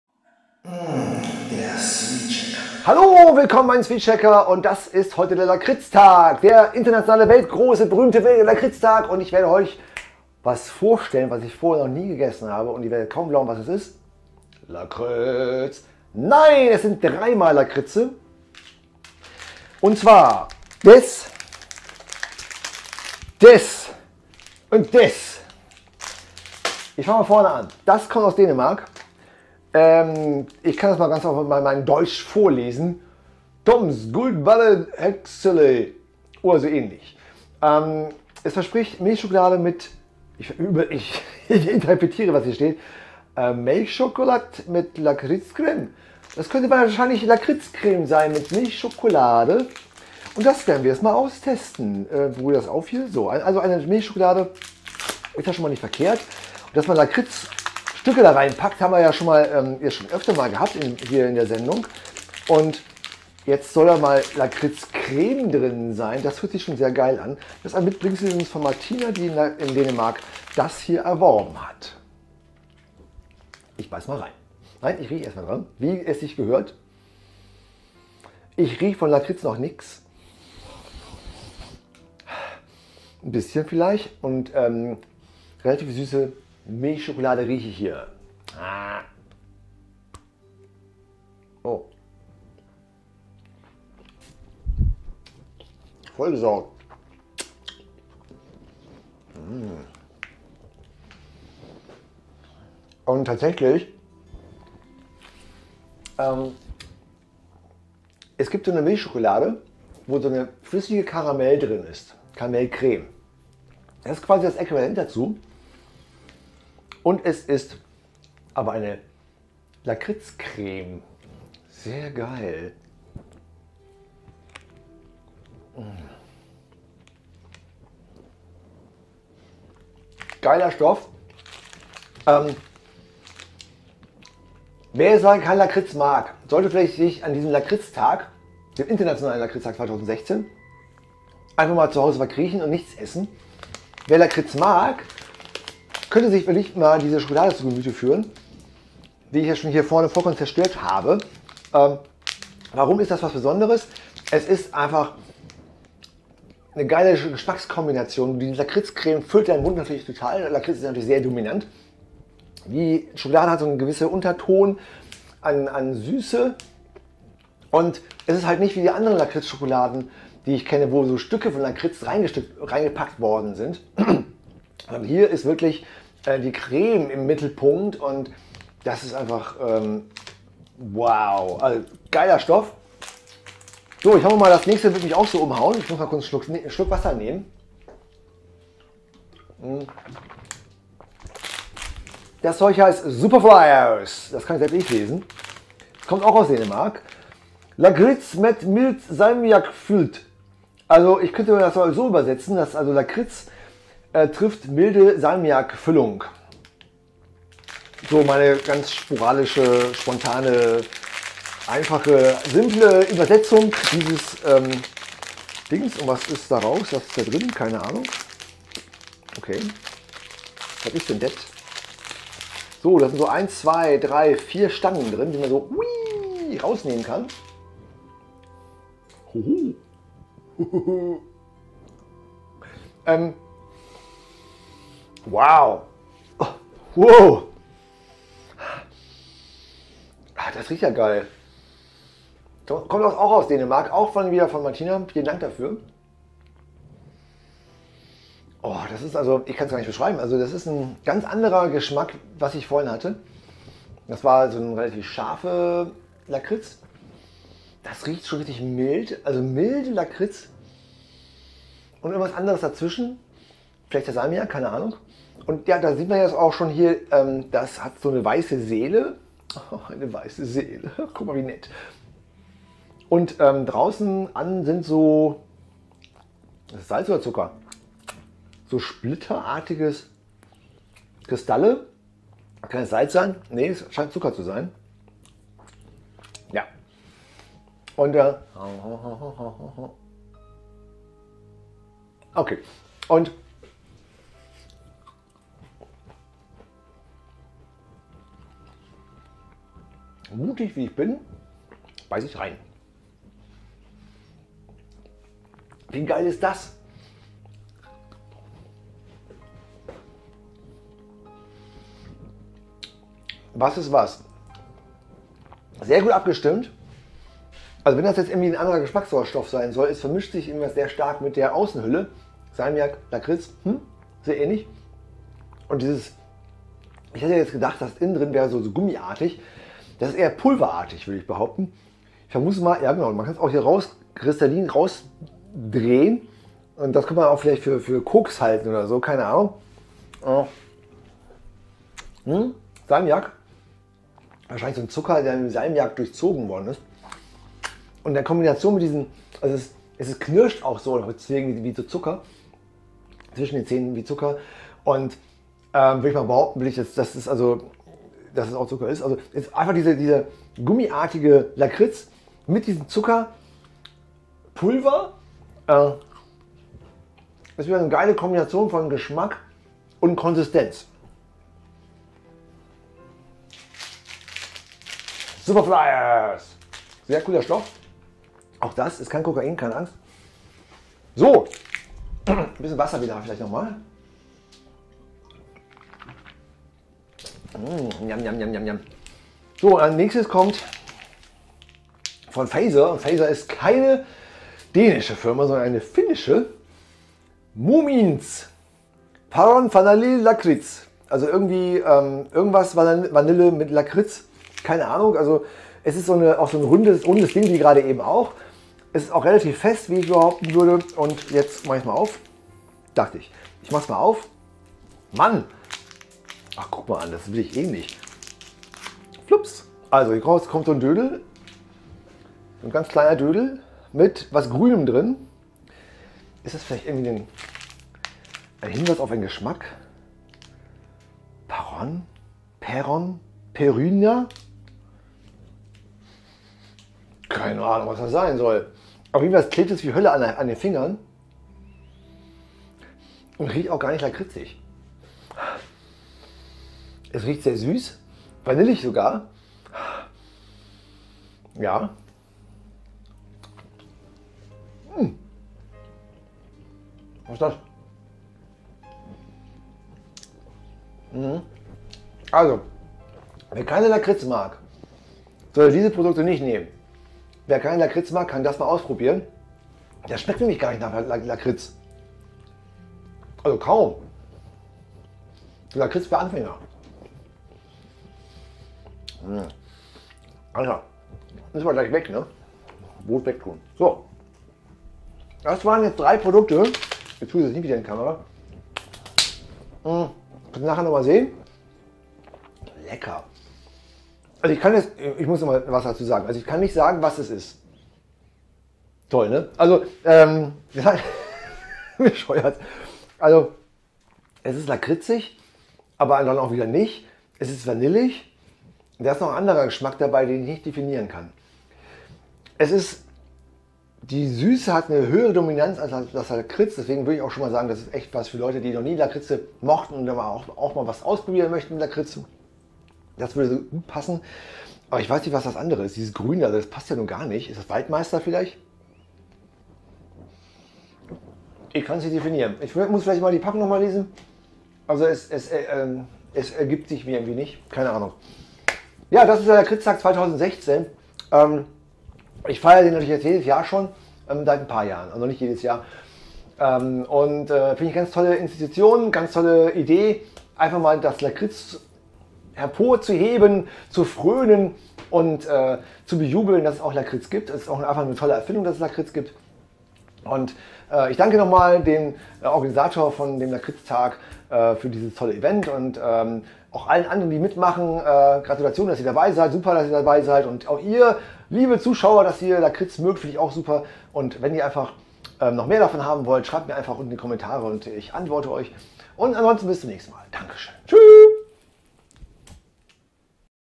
Lakritztag Spezial ♥ SweetsChecker ♥ Süßigkeiten Vorstellung und Test
Oh du Fröhlicher, oh du Seliger Lakritztag. Gnadenbringende schwarze Masse.

Jaaaaaa, heute der 12.April ist der internationale Lakritztag der hiermit gefeiert wird. Ja, mach mit. Zur Feier des Tages wird nicht eine Süßigkeit, nicht zwei, sondern gleich drei Süßigkeiten getestet. Eine leckere Schokolade mit Lakritz und zwei gefüllte Lakritzstangen. Schaut rein!
Mal wieder, alles Schmuggelware von Martina. Danke dafür!

Wer mehr über den Lakriztag erfahrne möchte, findet hier mehr Informationen

Die hier gezeigten Produkte, Markennamen oder eingetragenen Warenzeichen sind Eigentum des jeweiligen Herstellers. Meine hier gezeigte Meinung stellt lediglich eine subjektive Meinung dar.